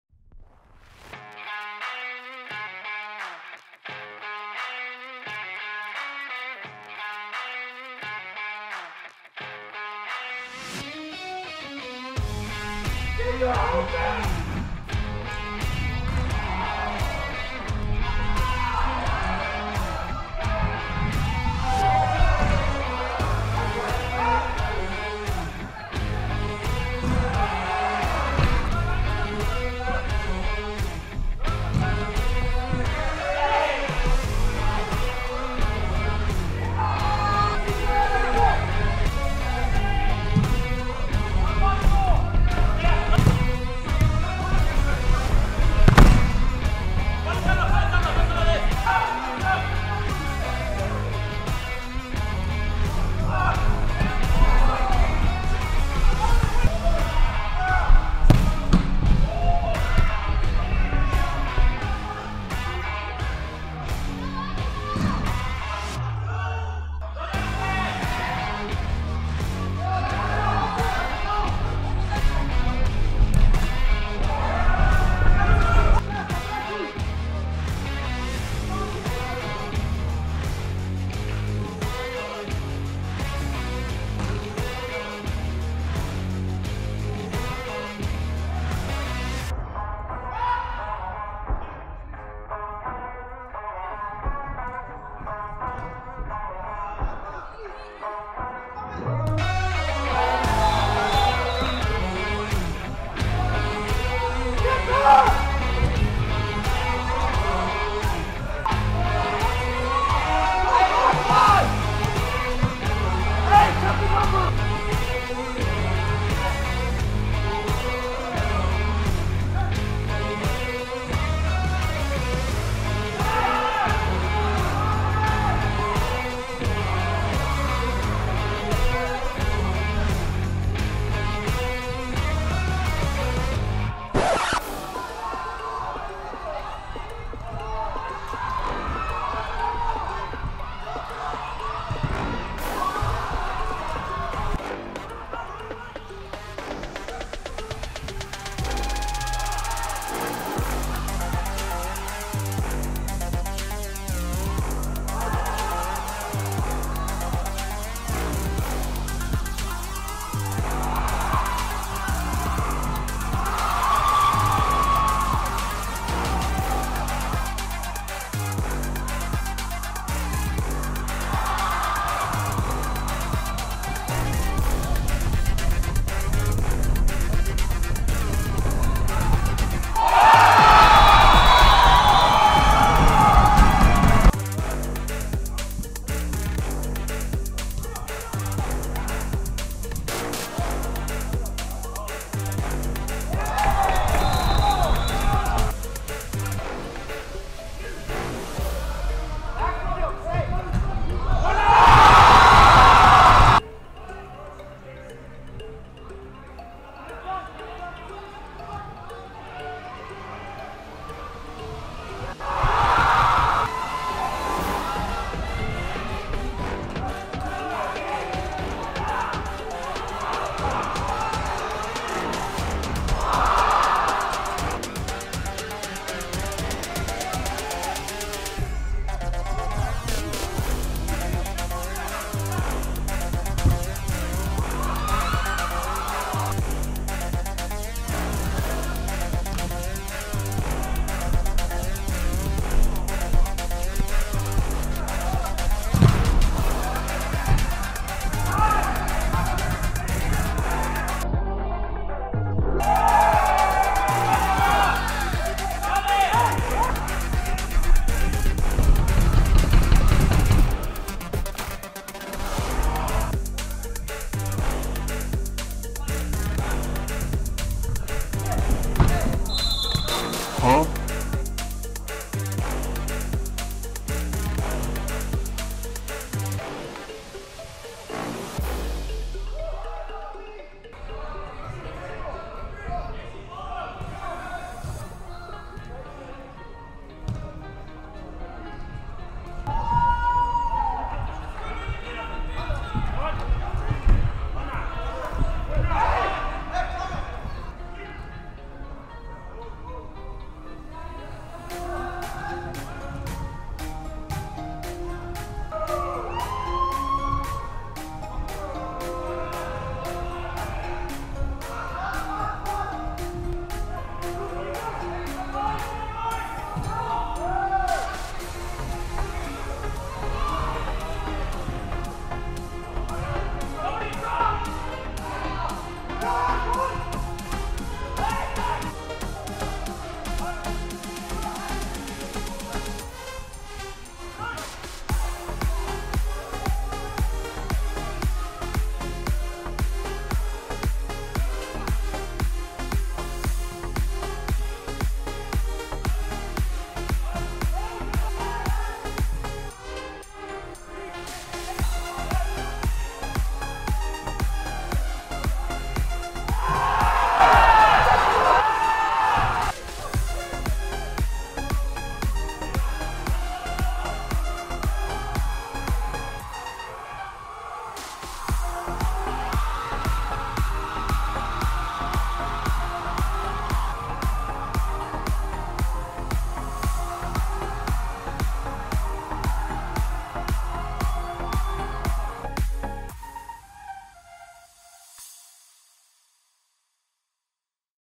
Michael Herrera’s 2022-2023 WISL highlights
Michael Herrera
Age: 22
Position: GK
Height 5’7
Weight: 160
Preferred foot: Right
College: Whatcom community college (Bellingham, Wa)
Club: Everett city fc
League: Western Indoor Soccer League (WISL)

Video credit: @lrider4809